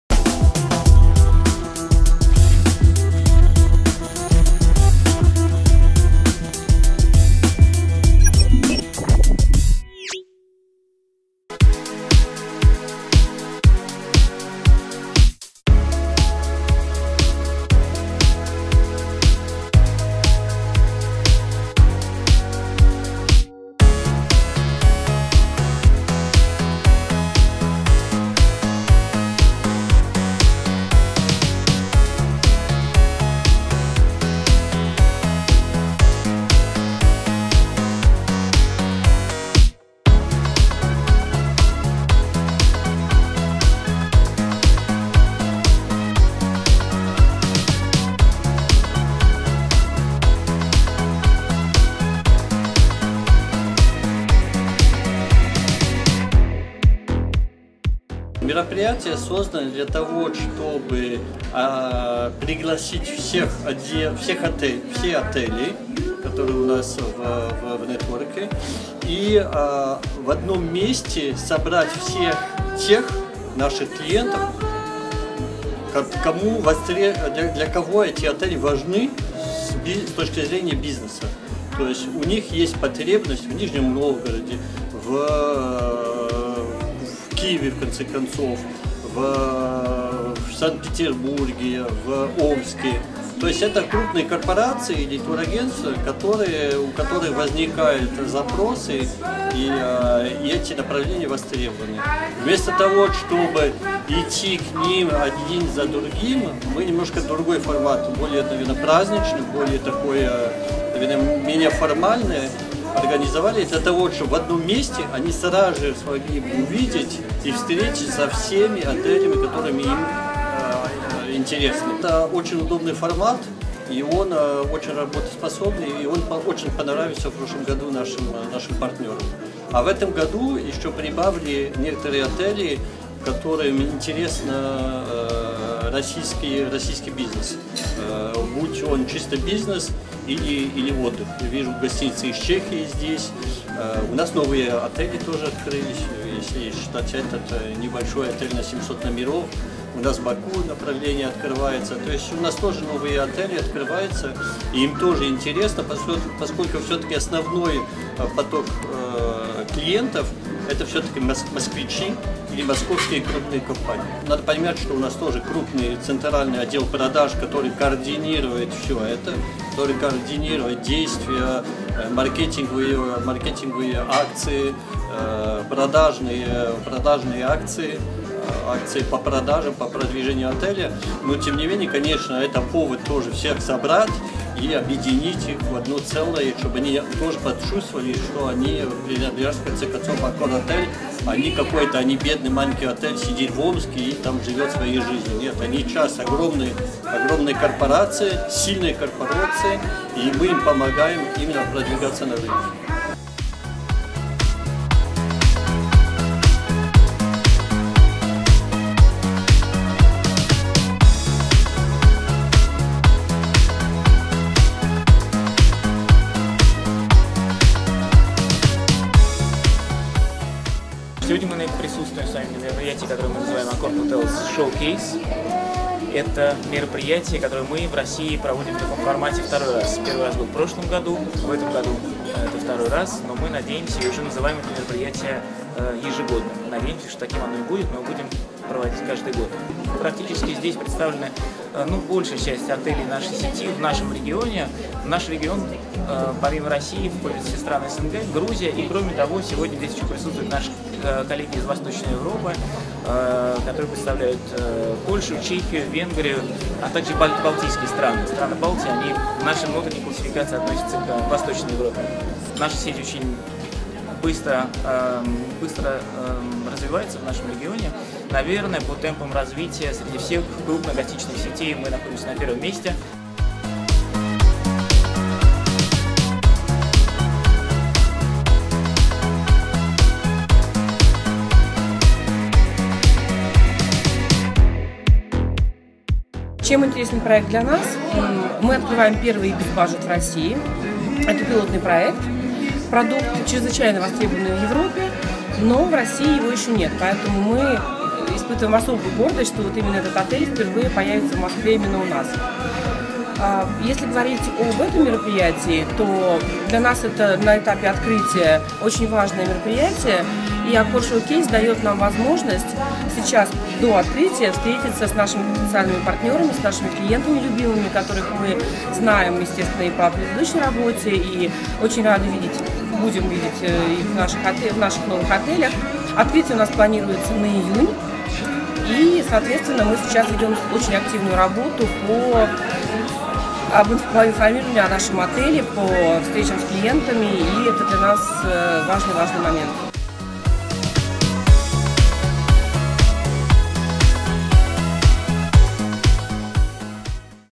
AccorHotels Showcase 2017. Место встречи восьми брендов с партнерами
Более 30 отелей из 16 стран бывшего СССР и Восточной Европы, 8 брендов AccorHotels собрались 26 апреля 2017 года для встречи с партнерами в рамках AccorHotels Showcase. Масштабное мероприятие, которое проходит уже второй год, состоялось в самом большом европейском гостиничном комплексе AccorHotels – ГК "Киевский", объединяющем в одном здании три гостиничных бренда – Novotel, ibis, Adagio.